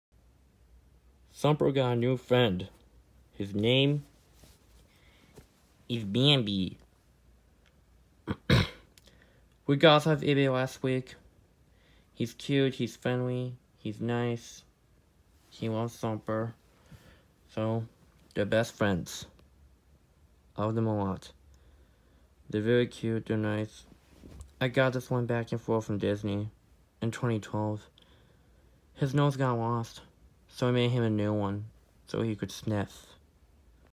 Today thumper got a friend named Bambi
Thumper and Bambi are best friends